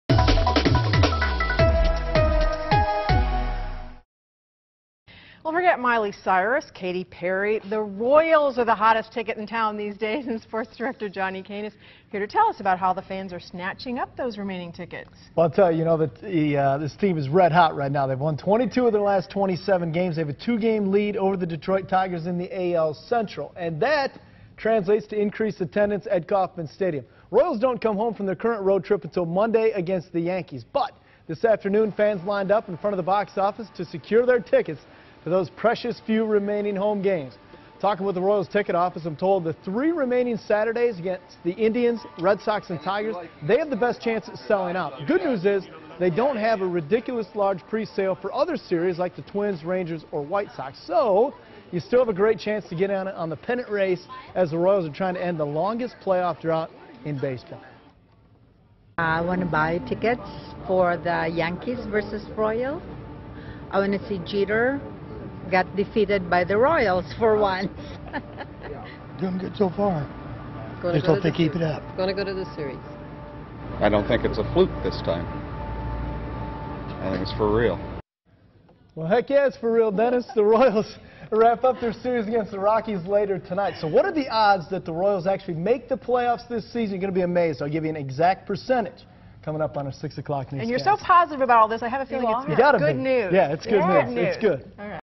Fans flock for tickets to see red-hot Royals
The first-place Kansas City Royals will be heading home next week in the thick of a playoff chase and fans lined up Wednesday to make sure they had tickets.